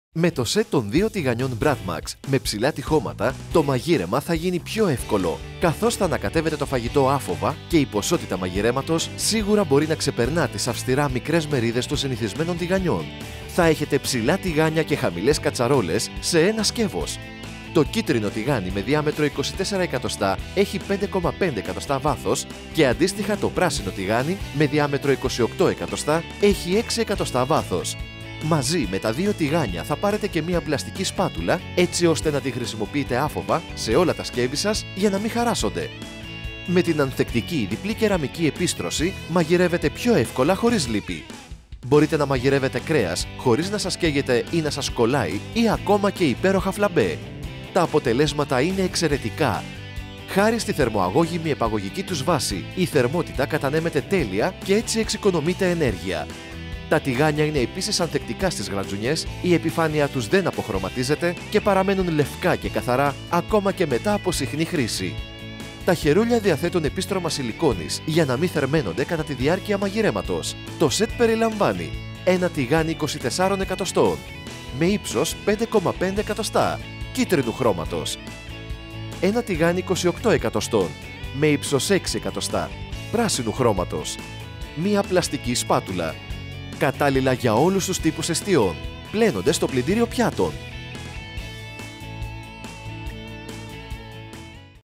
ΒΑΘΙΑ ΤΗΓΑΝΙΑ BRATMAXX ΜΕ ΠΛΑΣΤΙΚΗ ΣΠΑΤΟΥΛΑ (2 ΤΜΧ)
Τα βαθιά, αντικολλητικά τηγάνια για να απολαμβάνετε την μαγειρική.
Με τα τηγάνια Bratmaxx μπορείτε να μαγειρεύετε υγιεινά χωρίς λίπη και έλαια.

Χάρη στην ανθεκτική κεραμική τους επίστρωση το φαγητό δεν κολλάει και δεν καίγεται κι επομένως, δεν χρειάζεται να προσθέσετε επιπλέον έλαια.

Η επαγωγική τους βάση σας προσφέρει άψογη κατανομή θερμότητας και εξοικονόμηση ενέργειας.

Μπορείτε να μαγειρέψετε ό,τι επιθυμείτε στα τηγάνια, ακόμα και φλαμπέ και να τα καθαρίσετε στο λεπτό.
Η επιφάνειά τους δεν αποχρωματίζεται και είναι ανθεκτική στις γρατζουνιές ενώ παραμένει λευκή και καθαρή ακόμα και μετά από συχνή χρήση.

Τα χερούλια είναι από σιλικόνη για να μην θερμαίνονται κατά τη διάρκεια της μαγειρικής.
Είναι ανθεκτικά σε θερμοκρασίες έως 1500 C ενώ το τηγάνι είναι θερμικά ανθεκτικό έως τους 2800 C.

Κατάλληλο για όλους τους τύπους εστιών.

Πλένονται στο πλυντήριο πιάτων.

Το σετ περιλαμβάνει:
- 1 τηγάνι 24 εκ με ύψος 5,5 εκ.
- 1 τηγάνι 28 εκ με ύψος 6 εκ.
- 1 σπάτουλα πλαστική